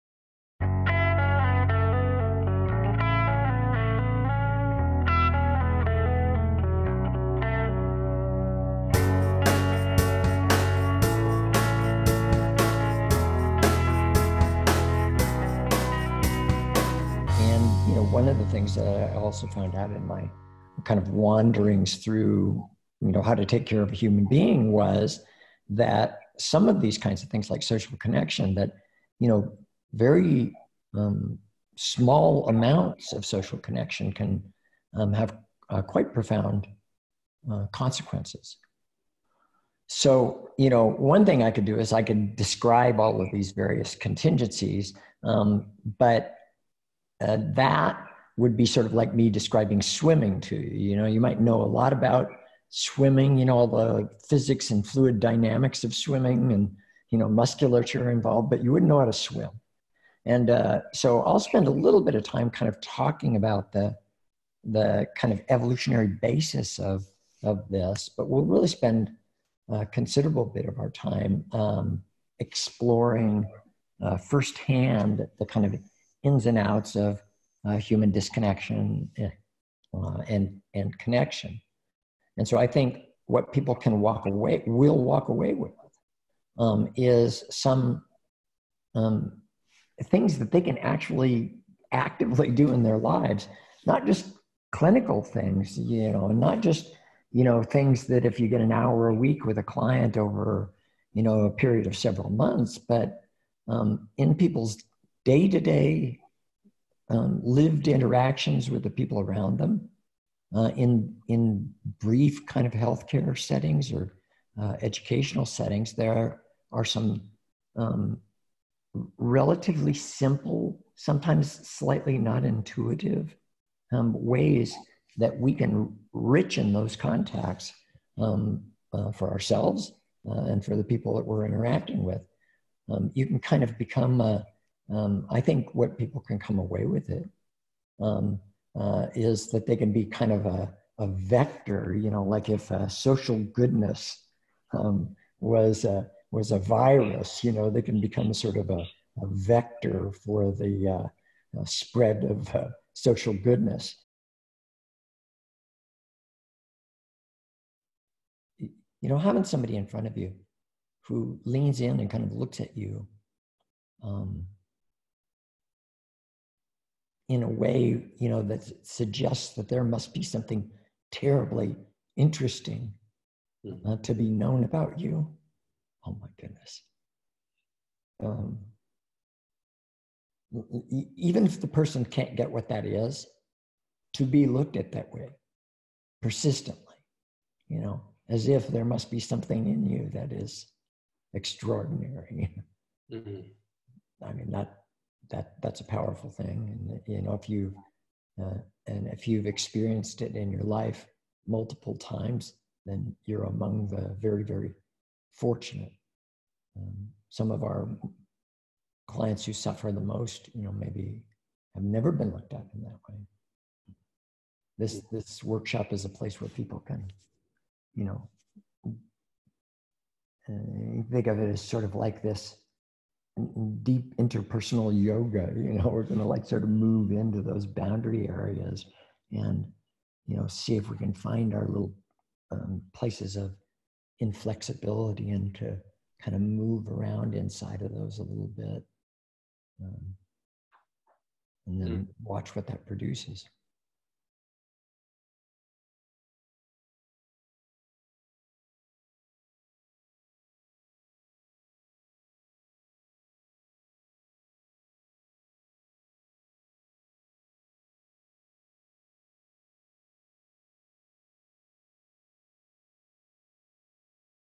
Kelly Wilson on his workshop The Therapeutic Relationship in ACT & Evolution Science NY Feb 2020
An introduction and invitation to Kelly Wilson's Workshop on The Meaning & Development of the Therapeutic Relationship: ACT & Evolution Science on Feb 7 & 8 2020. See Eventbrite.com and search Kelly Wilson or go